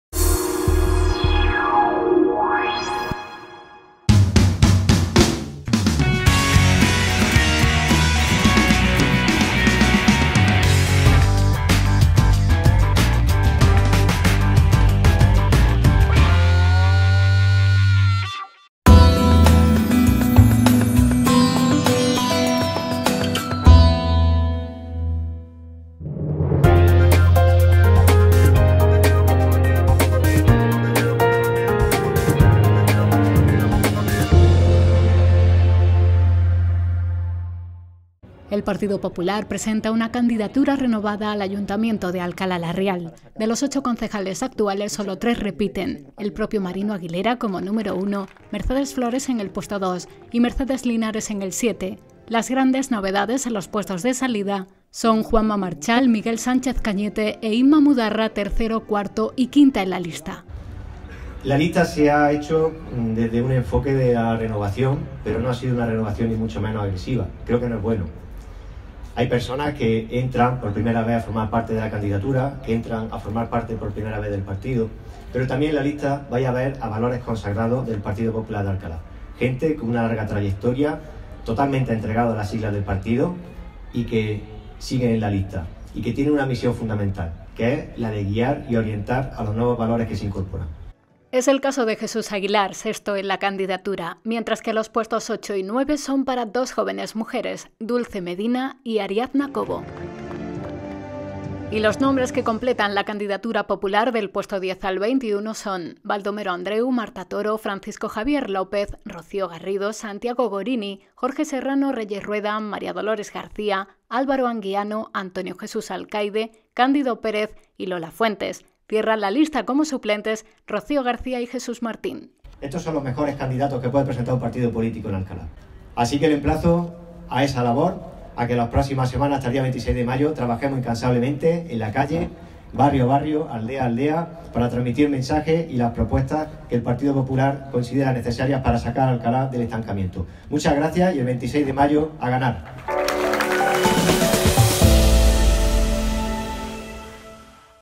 Presentación Candidatura PP Alcalá la Real
El Partido Popular presenta una candidatura renovada al Ayuntamiento de Alcalá la Real. De los 8 concejales actuales, sólo 3 repiten: el propio Marino Aguilera, como número 1, Mercedes Flores, en el puesto 2 y Mercedes Linares, en el 7. Las grandes novedades en los puestos de salida son Juanma Marchal, Miguel Sánchez Cañete e Inma Mudarra, tercero, cuarto y quinta en la lista.

“Aunque la lista se ha hecho desde un enfoque de renovación, no se trata de una renovación agresiva, ya que no es bueno. Hay personas que entran a formar parte por primera vez de la candidatura e incluso del partido, pero también cuenta con valores consagrados del Partido Popular de Alcalá, y cuya función será la de guiar y orientar a los nuevos valores que se incorporan” asegura Aguilera.

Es el caso de Jesús Aguilar, sexto en la candidatura, mientras que los puestos 8 y 9 son para dos jóvenes mujeres, Dulce Medina y Ariadna Cobo.

El resto de nombres que completan la candidatura popular del puesto 10 al 21 son: Baldomero Andréu, Marta Toro, Francisco Javier López, Rocío Garrido, Santiago Gorini, Jorge Serrano, Reyes Rueda, Mª Dolores García, Álvaro Anguiano, Antonio Jesús Alcaide, Cándido Pérez y Lola Fuentes. Cierran la lista como suplentes Rocío García y Jesús Martín.

Marino Aguilera ha tenido además palabras de agradecimiento para los concejales que le han acompañado durante la legislatura que en breve termina y ha remarcado que presenta "a un grupo de personas que no vienen a la política a conseguir algo que ya tienen y han logrado fuera. Personas que se han trabajado a sí mismas y que no le deben nada a nadie”.